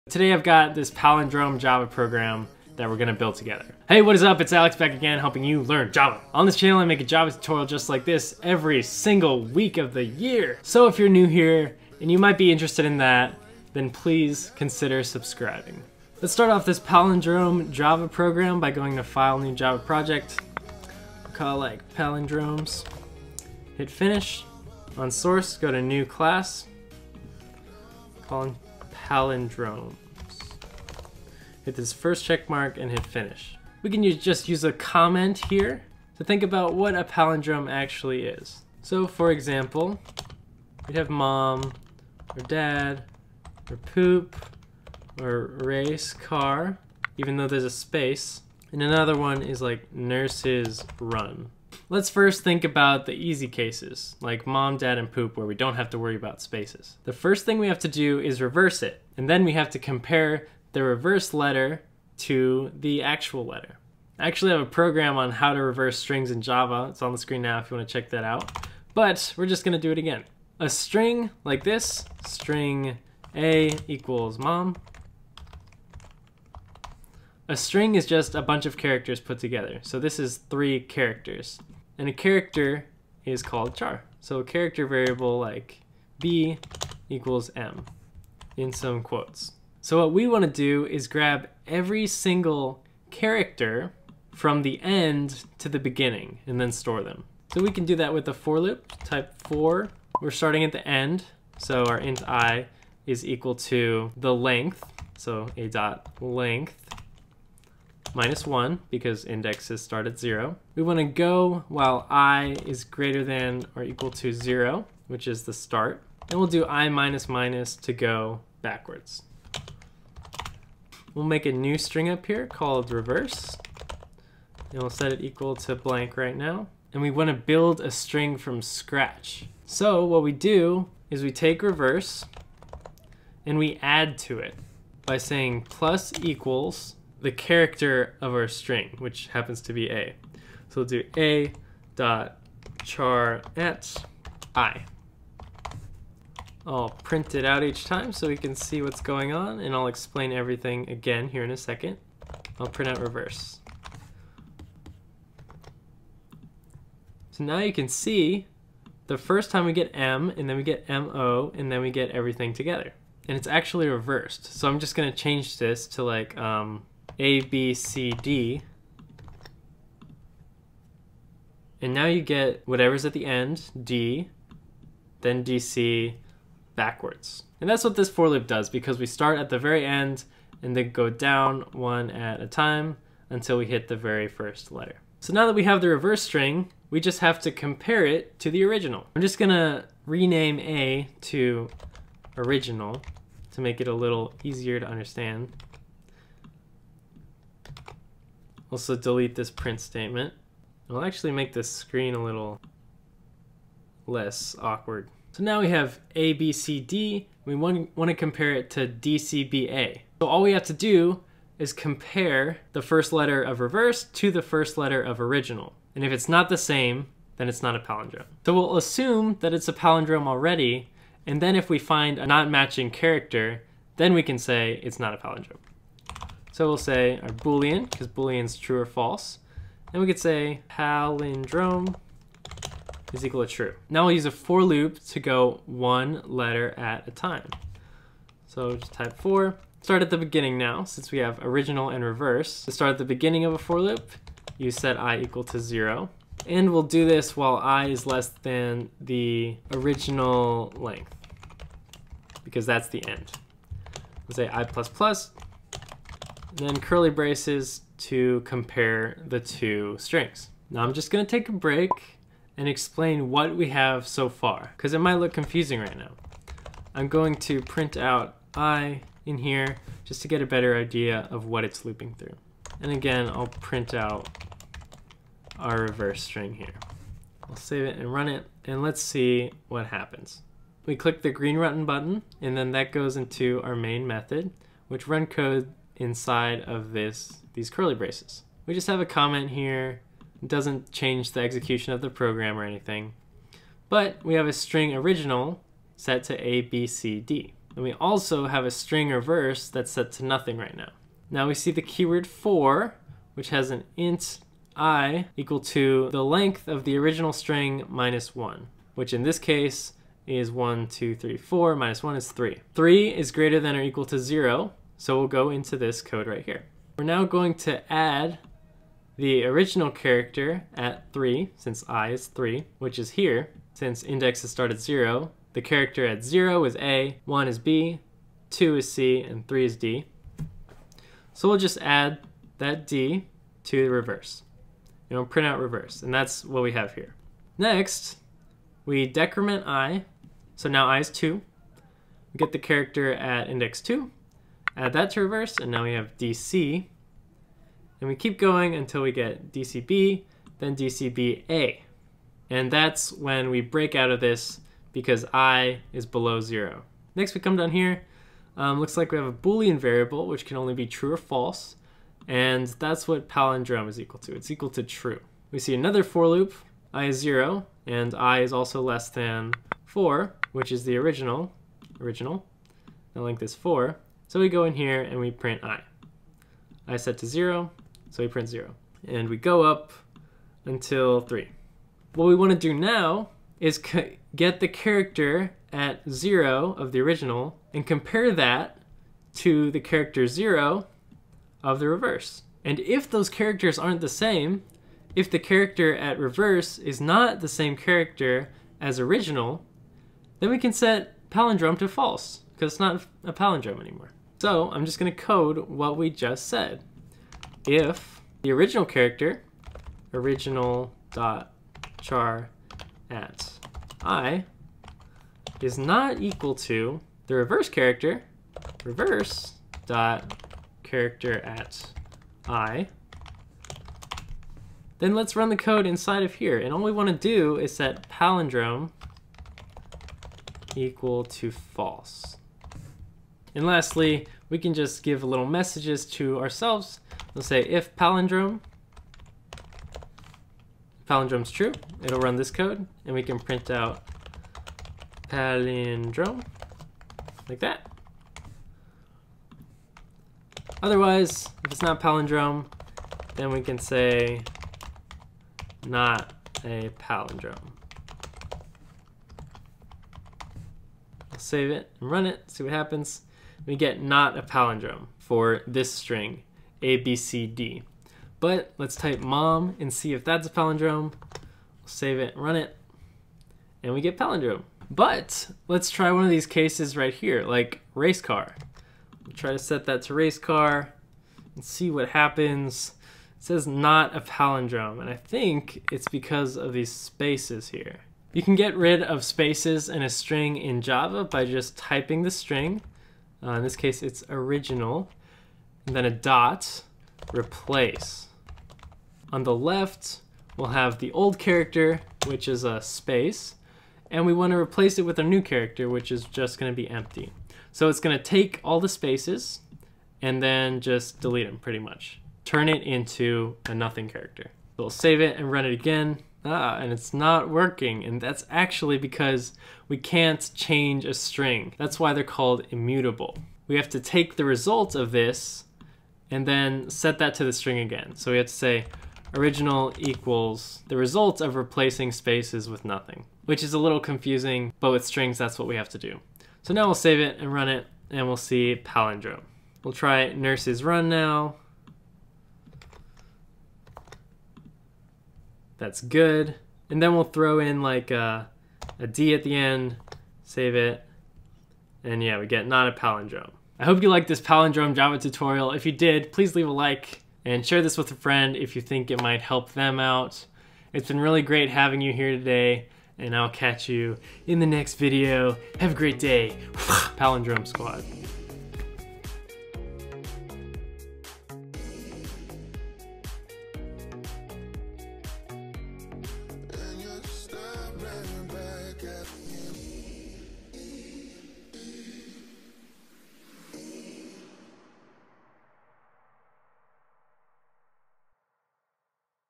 Palindrome Program In Java Tutorial #63
$1,500 OFF ANY Springboard Tech Bootcamps with my code ALEXLEE1500. See if you qualify for the JOB GUARANTEE!

This palindrome program in java and explanation will help you get working code! ✅Hopefully, what I've taught you will help you with your palindrome java program.

A palindrome is a word or phrase that is the same reversed, as it is originally:

mom
dad
race car

If you reverse each of these (and exclude spaces) then they will spell the same word. In this palindrome java program, we will use a for loop to get each character from the word, and then use another for loop to compare each reversed character to the original to determine if the String is indeed a palindrome.

This palindrome java code can be tricky at first... But SURELY you'll get it :) If you followed along, congrats! You learned-by-doing!

I hope you enjoyed this palindrome program in java tutorial! I like to have a nice mix of tutorials and actual projects for you all :)

Do you want to learn how to write java code from scratch, in Java?

Was this able to help you find palindromes in java?

Alex Lee